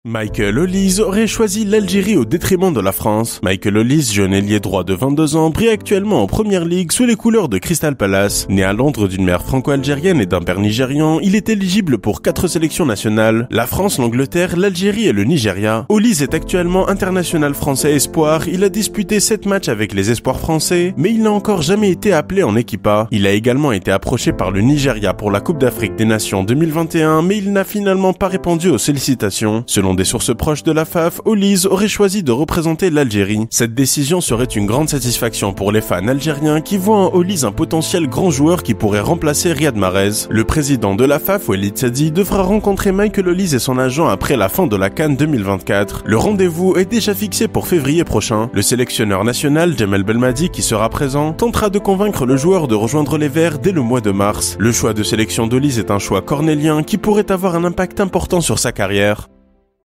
Amine Gouiri : Polémique entre Djamel Belmadi et le Stade rennais
La blessure inattendue de l'attaquant international algérien, Amine Gouiri et son forfait pour la Coupe d'Afrique des nations (CAN 2024) en Côte d'Ivoire ne passent pas, vu le reproche fait par le sélectionneur national Djamel Belmadi aux dirigeants du Stade rennais où évolue le joueur, notamment en lien avec le manque de communication du club breton sur la blessure de l'attaquant algérien. La réaction du Stade rennais aux propos de Djamel Belmadi est ferme mais semble vouloir éviter la polémique.